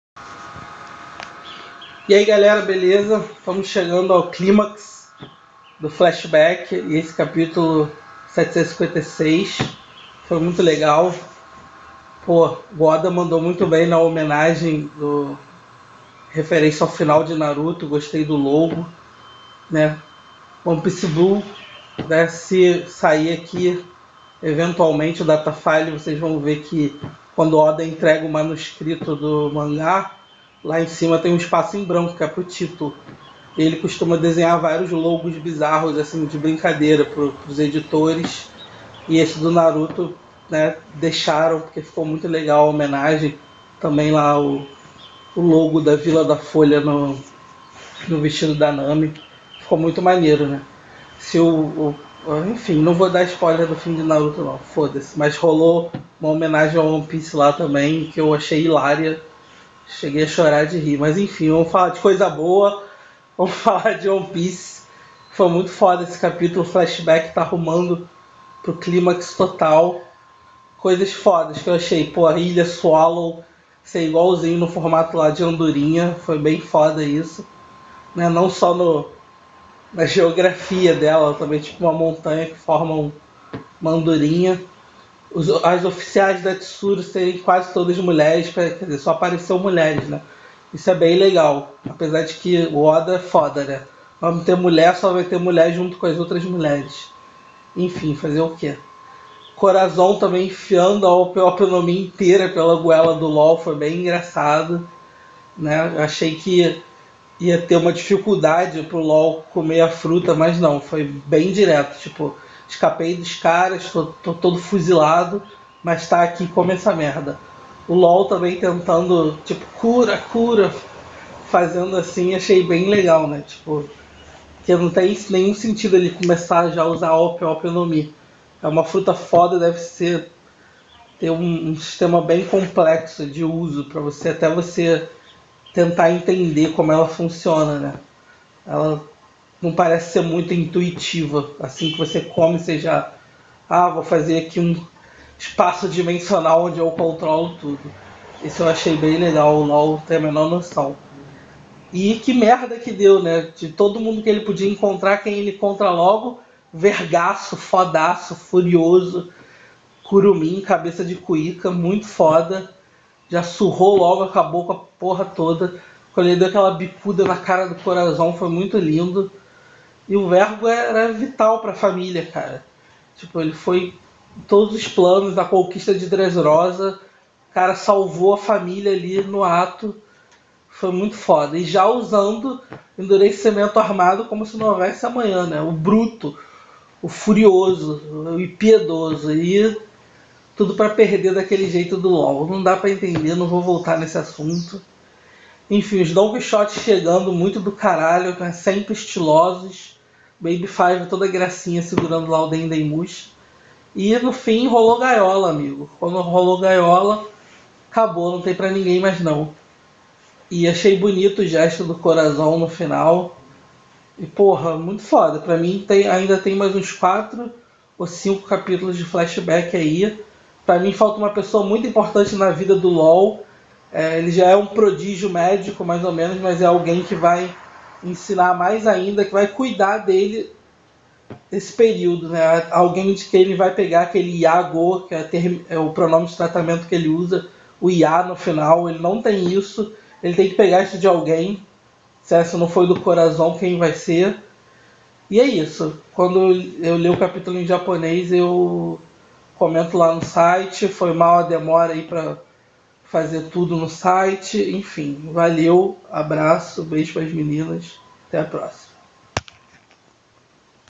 RDTV Primeiras Impressões: Cap. 766! MASSACRE ao Traidor!!
Flashback chegando o clímax!! HAJA CORAZÓN, AMIGOS!!